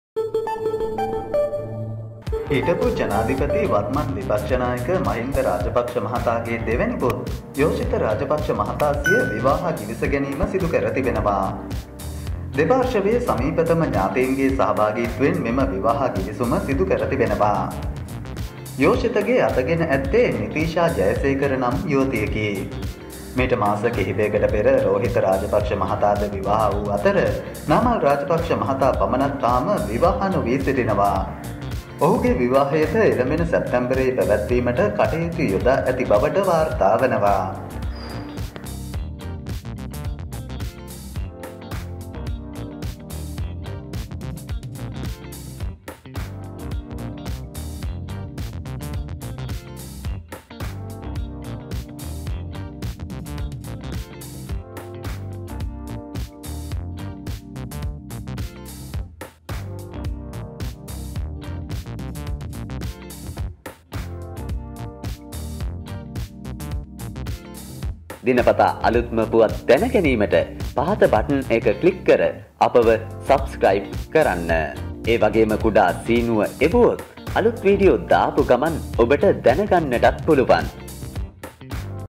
යෝශිතගේ අත ගත් සුරූපී තරුණිය මෙන්න.. යෝෂිත රාජපක්‍ෂට සුභ මංගලම්..
යෝෂිත රාජපක්‍ෂට සුභ මංගලම්.. විවාහ ගිවිස ගනී..
මහින්දගේ දෙවැනි පුතත් විවාහ වෙයි.. බොහෝ දෙනෙක් නොදුටු එම උත්සවයේ ඡායාරුප මෙන්න